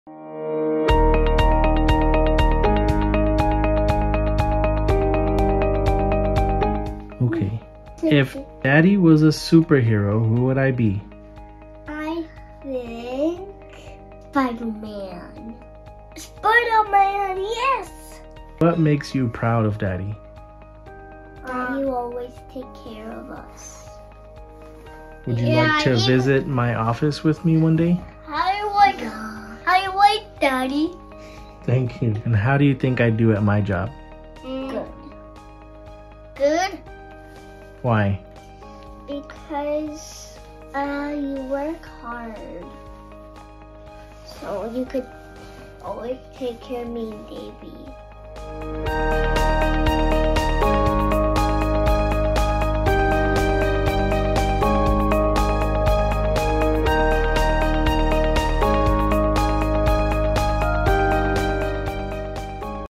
Celebrating DayForDad with Jose and his little fans
Cheers to the man who made life’s hard points easy. This DayForDad is to thank all the fathers for their love and affection. Anabelle and Davian, children of Jose - Team Leader Operations, HGS, are here to share the vibes of love and affection of their daddy.
Share with us your children’s words to win and get featured too!